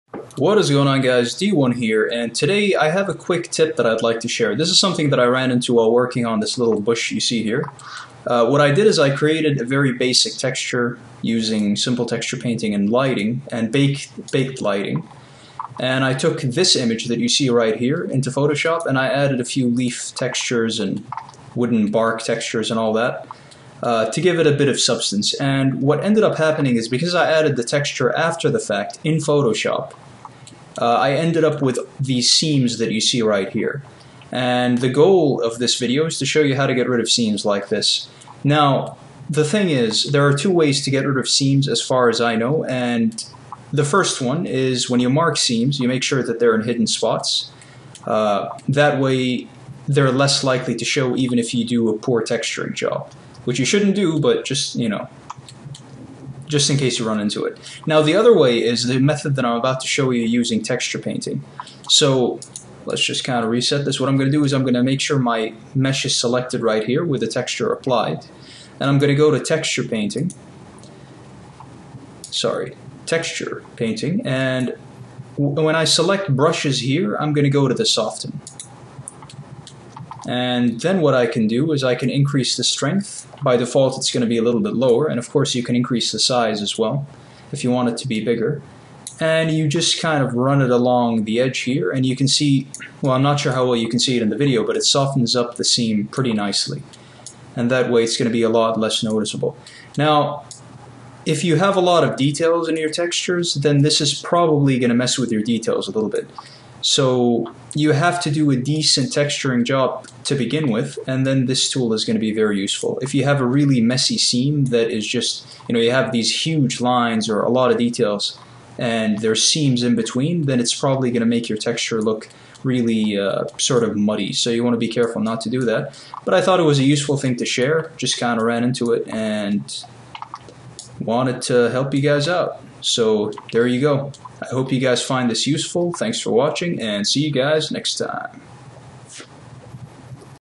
Blender Quick Tip: How to remove seams in your textures
Hey guys! Just a quick tip for you guys that shows a method I discovered for getting rid of seams. Blender's texture painting is better than I thought after all. :)